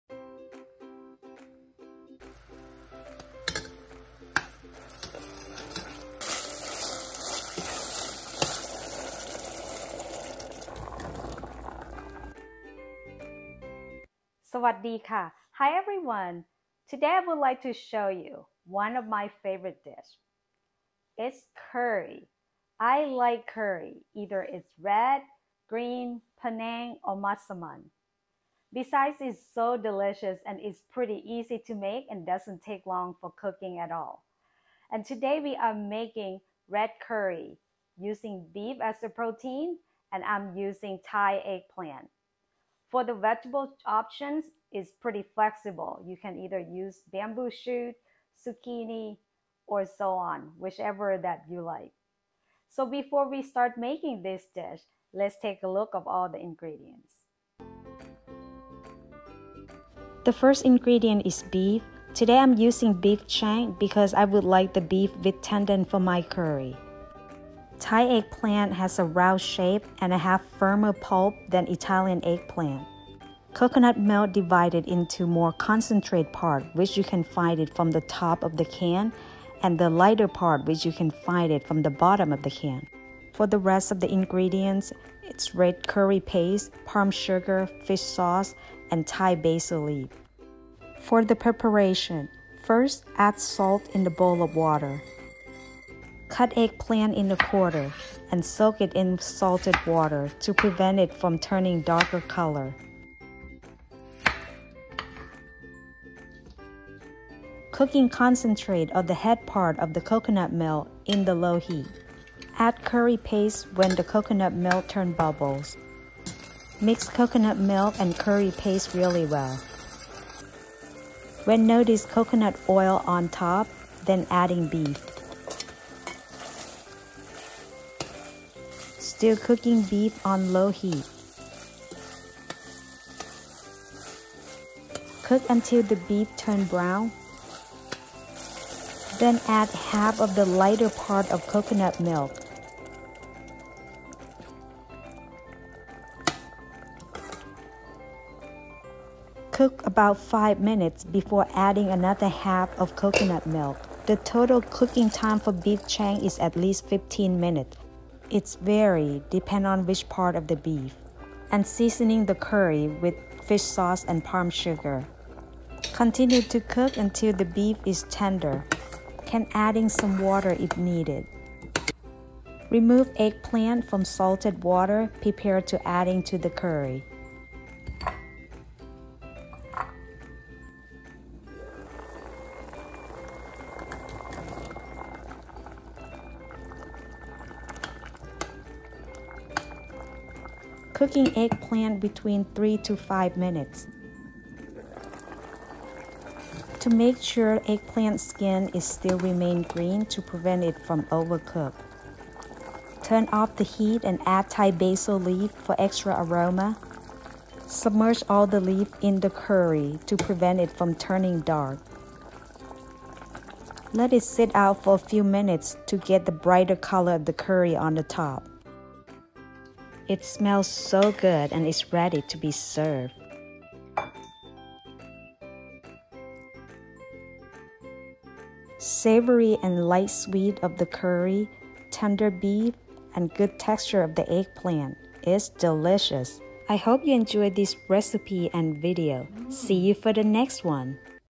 EP.122 Thai Beef Red Curry!! Savory and light Sweet Curry with Tender Beef & delicious Egg Plant!!
delicious Thai red curry# tender beef# good texture egg plant👍#